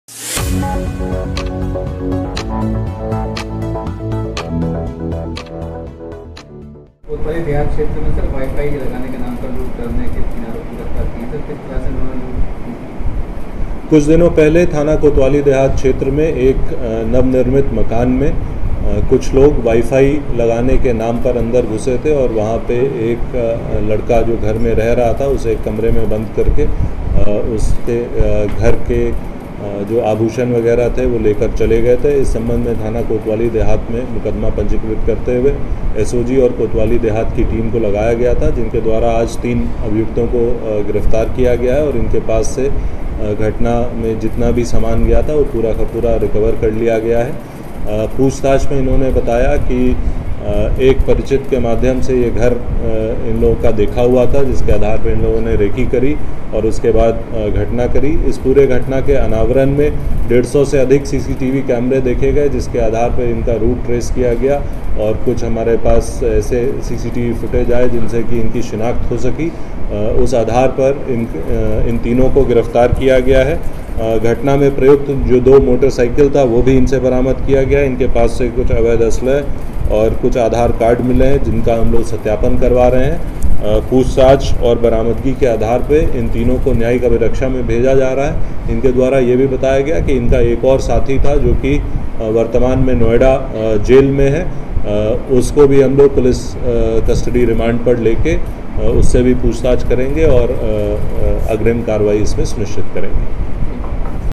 Bulandshahr Police को मिली बड़ी कामयाबी | 3 शातिर लूटेरों को किया गिरफ्तार | UP Crime News
Bulandshahr Police को मिली बड़ी कामयाबी | 3 शातिर लूटेरों को किया गिरफ्तार |  UP Crime News | @delhiglownews
थाना केतवाली देहात पुलिस व स्वाट टीम द्वारा लूट की घटना कारित करने वाले  03 शातिर लुटेरे गिरफ्तार, कब्जे से लूटे गये आभूषण, नगदी, घटना में प्रयुक्त मोटरसाइकिल, अवैध असलहा, कारतूस आदि बरामद।